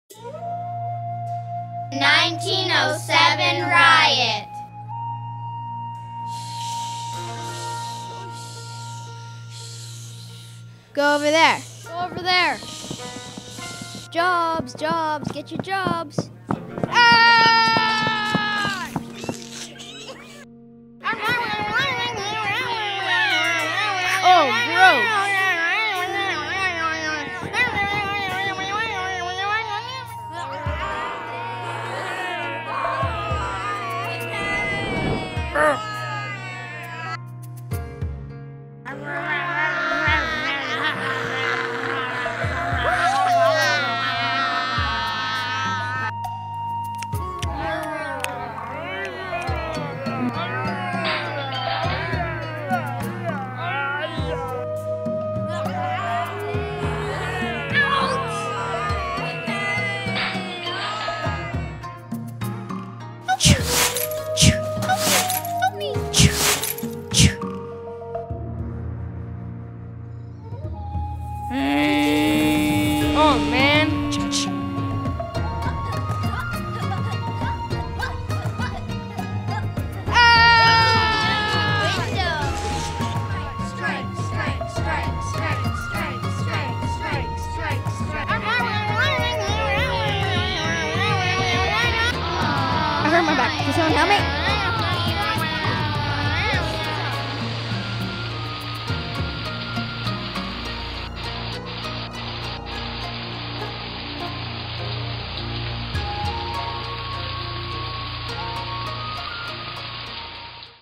1907 Riot -- Queen Victoria Annex (December 10, 2013)
This short was produced on December 10, 2013 during a one-day workshop called Animating History, offered at the Museum of Vancouver in partnership with the Reel 2 Real International Film Festival for Youth. Participants worked cooperatively to storyboard the film's plot, create characters, animate their creations using computer-operated digital cameras, and add dialogue and sound effects.

In Vancouver on September 1907, hundreds of angry Caucasians attacked local residents and smashed storefront windows in Chinatown and Japantown. The riot took place after a march organized by the Asiatic Exclusion League that wound up in a rally at City Hall, which was then located on Main Street. With banners reading Stand for a White Canada, the demonstrators, including politicians and labour leaders, called for an end to Asian immigration to British Columbia.